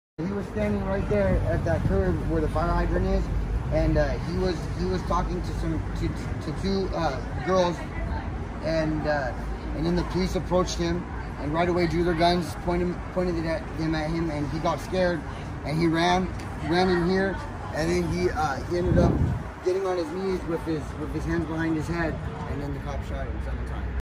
El Manager, Andrew Haney Cuenta Como Mataron a Andres Guardado
Andres Guardado había venido a hablar con el gerente
corrió por el camino de entrada, se arrodilló y se llevó las manos a cabeza
Entonces un policía le disparó en la espalda7 VECES
después LAPD sacó una orden judicial y rompió todas las cámaras
también se llevaron el DVR